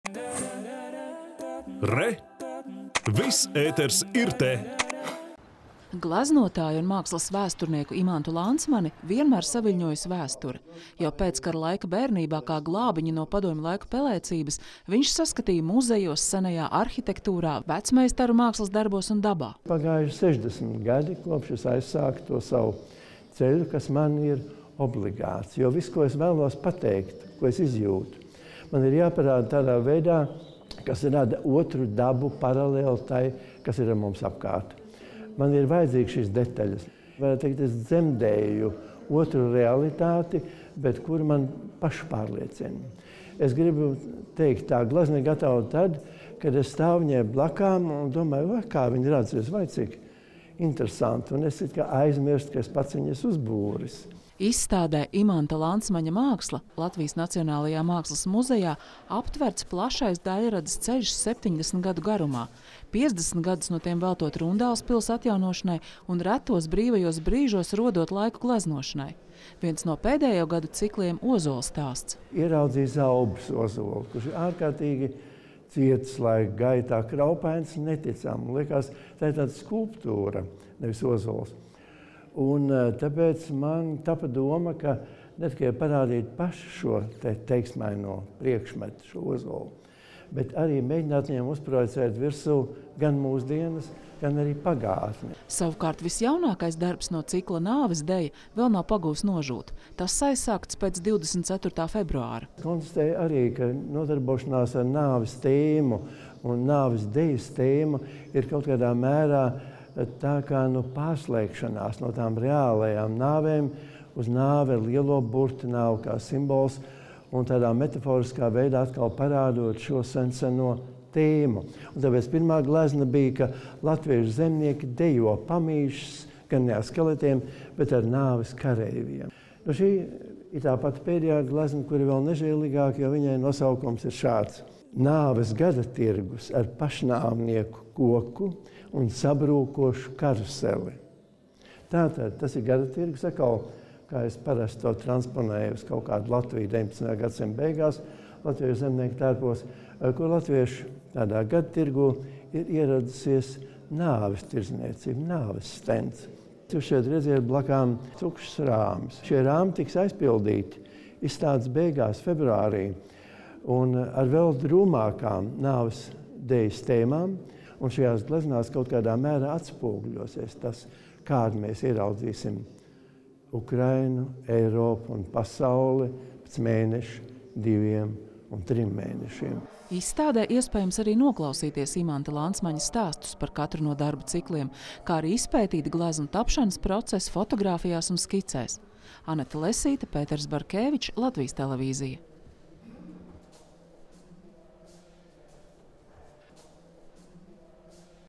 Imanta Lancmaņa bagātīgā māksla septiņdesmit gadu šķērsgriezumā
Pērn mākslinieks un mākslas vēsturnieks Imants Lancmanis svinēja astoņdesmito jubileju. Piecdesmit gadi no tiem aizvadīti, glābjot un atjaunojot Rundāles pili, un vienlaikus arī gleznojot. Rīt Latvijas Nacionālajā mākslas muzejā durvis vērs retrospektīva izstāde “Imanta Lancmaņa māksla”. Ar leģendāro meistaru tikās  Anete Lesīte.